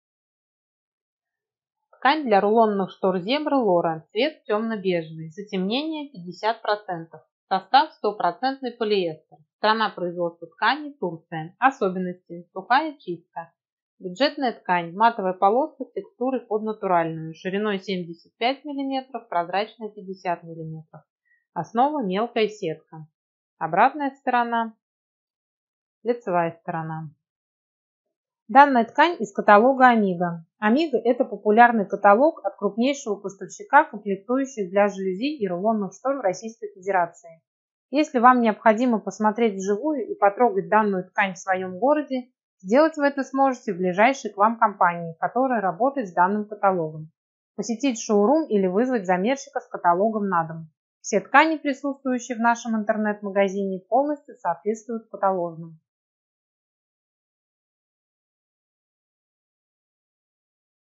Лора Тёмно-бежевый Амиго Зебра
Ткань для рулонных штор Зебра - Лора. Цвет - Тёмно-бежевый. Затемнение - 50%. Состав - Полиэстер-100%. Страна производства ткани - Турция. Особенности - Сухая чистка. Бюджетная ткань. Матовая полоска с текстурой под натуральную - шириной 75 мм, прозрачная 50 мм. Основа - мелкая сетка . Данная ткань из каталога Амиго - Зебра. Купить ткань или рулонные шторы Зебра с этой тканью Вы сможете в видео интернет-магазине ЖАЛЮЗНИК